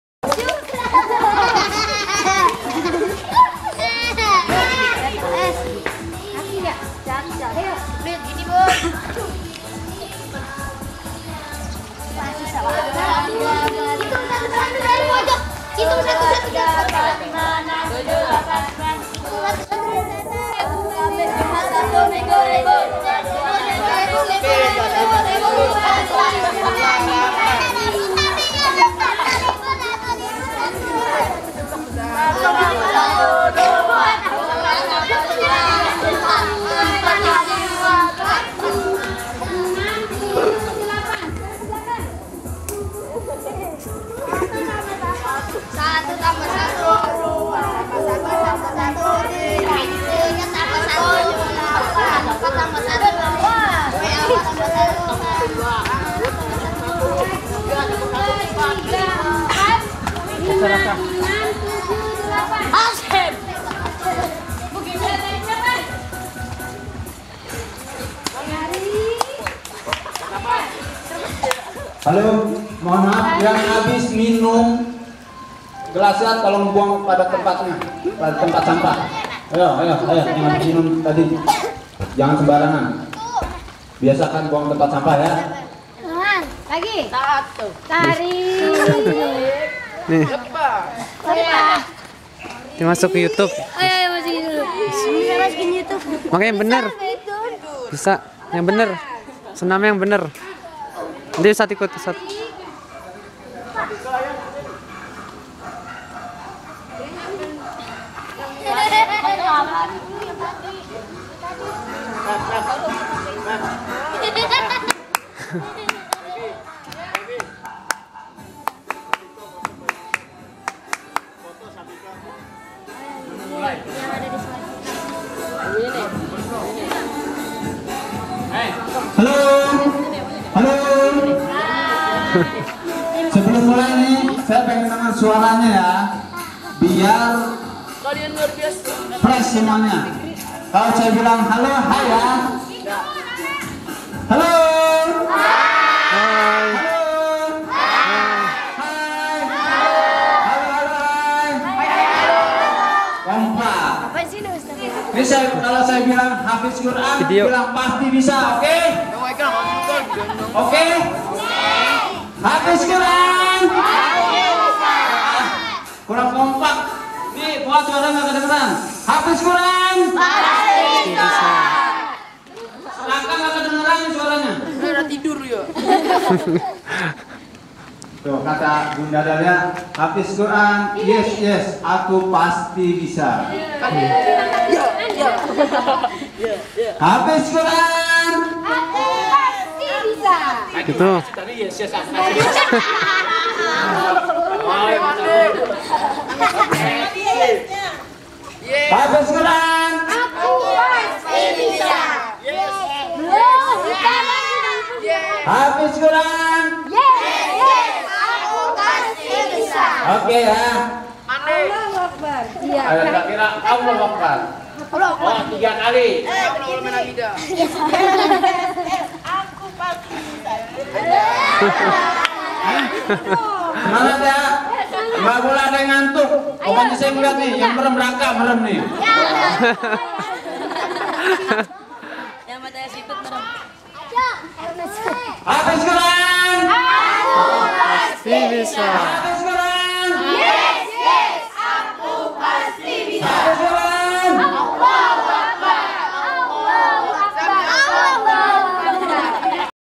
Senam Pagi di Sela-sela Karantina Hafalan Al-Qur'an ~ Rumah Qur'an Daru Hasbi
Villa Kota Bunga, Cipanas - Puncak Bogor.
15 Oktober 2016